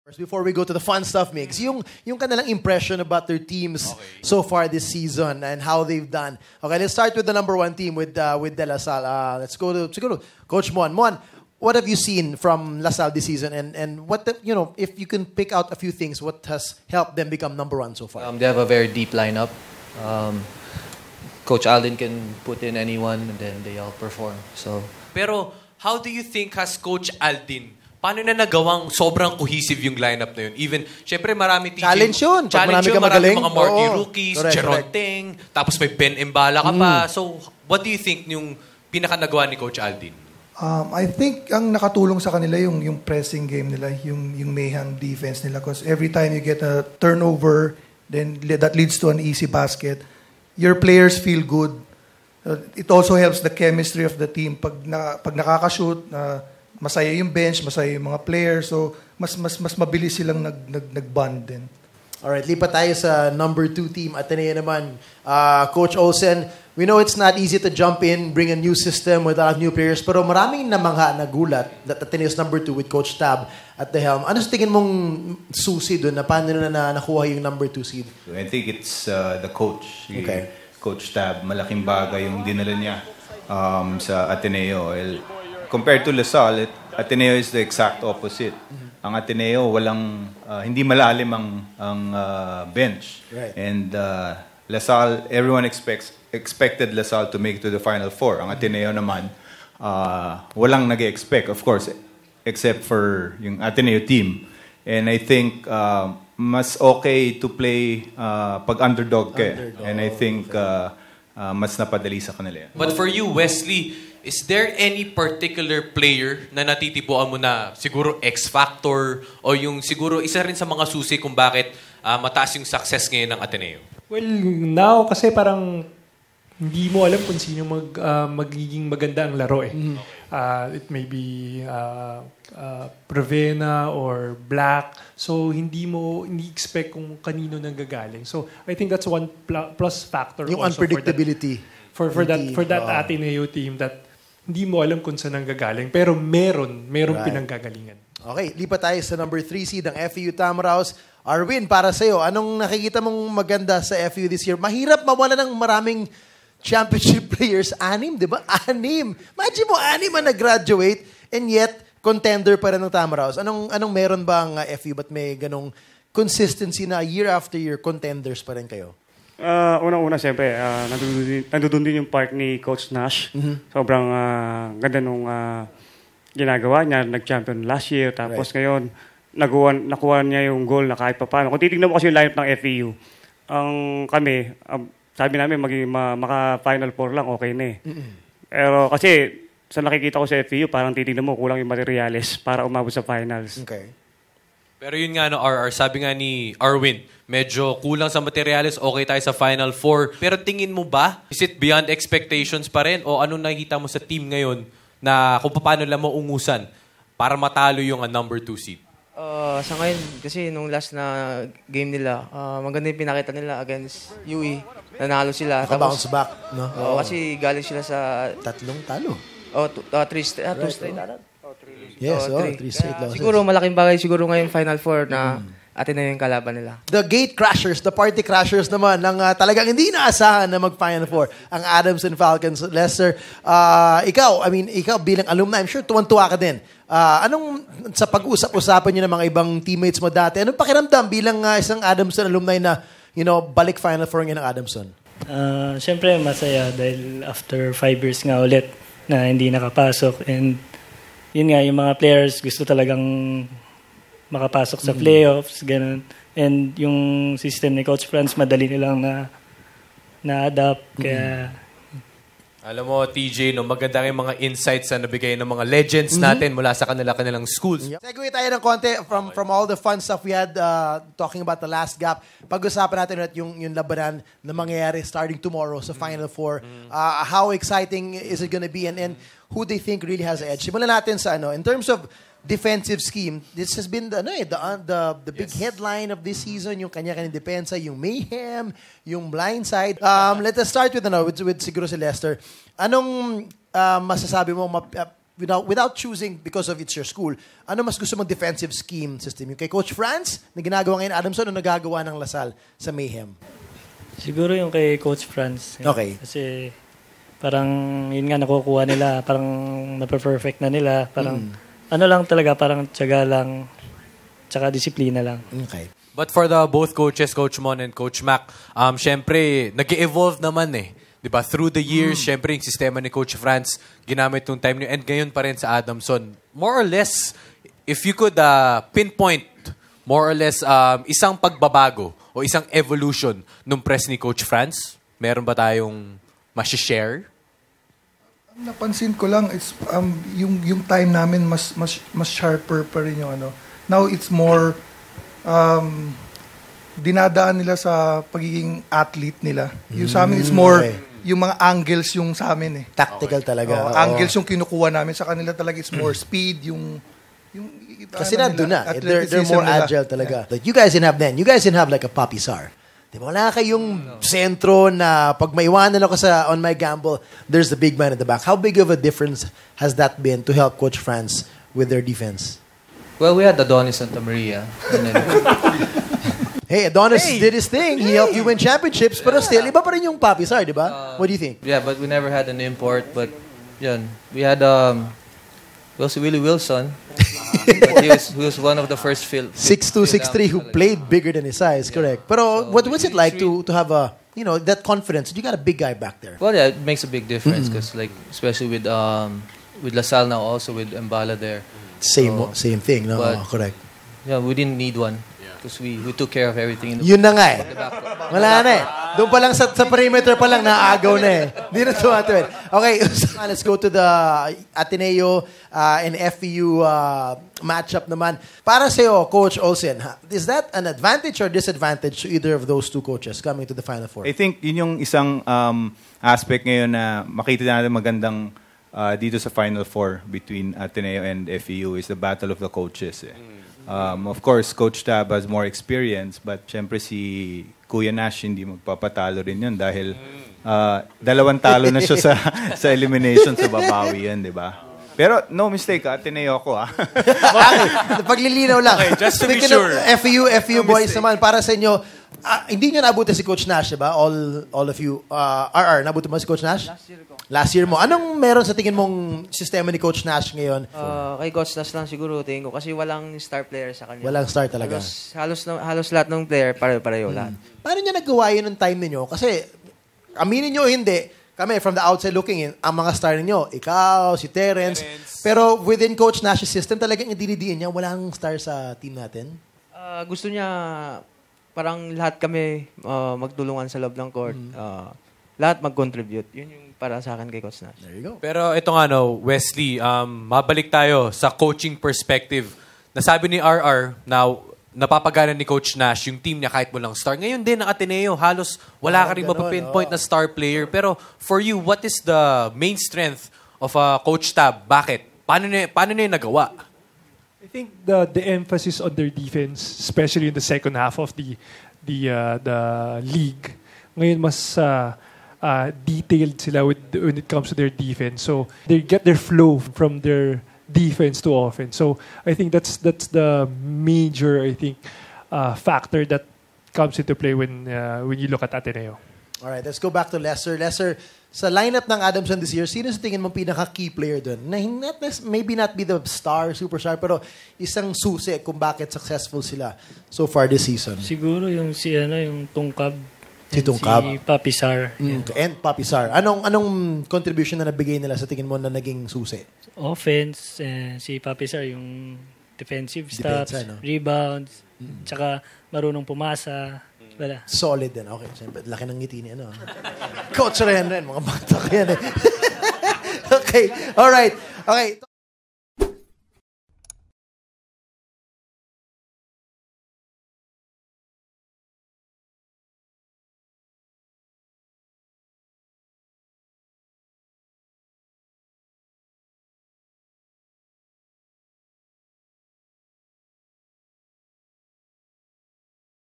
Final Four analysis of the UAAP Legends | S+A Unfiltered: The UAAP Final Four Special
With the men's seniors basketball Final Four of UAAP Season 79 already in place, we found it timely to ask a handful of former UAAP players from past decades to weigh in on this season's tournament picture, and one of the questions that were asked of them was who they could compare themselves to in the league today.

These UAAP legends weighed in on a whole lot more, including rivalries, social media, and even pre-game outfits!